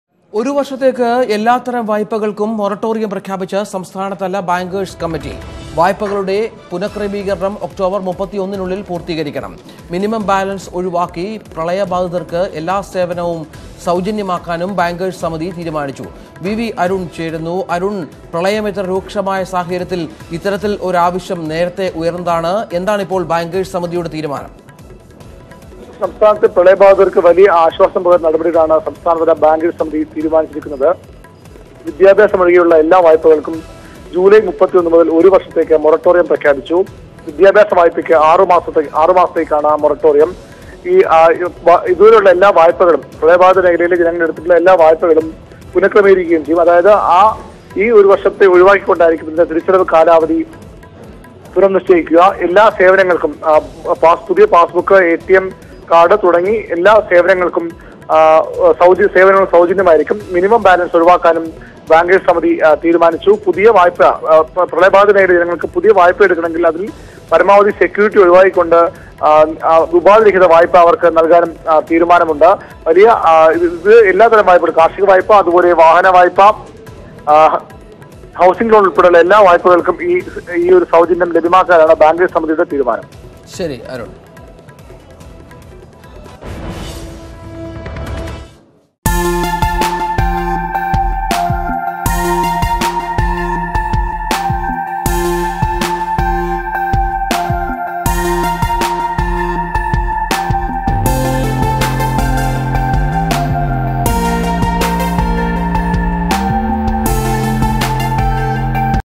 കേരളത്തിൽ ഒരു വർഷത്തെ Moratorium പ്രഖ്യാപിച്ച് സംസ്ഥാനതല ബാങ്കേഴ്സ് കമ്മിറ്റി | 22nd August 2018
The State Level Banker's Committee decided to put moratorium on loans. This moratorium has been declared considering the situation in the state, Moratorium is included for all types of loans which has been taken by the people of the state.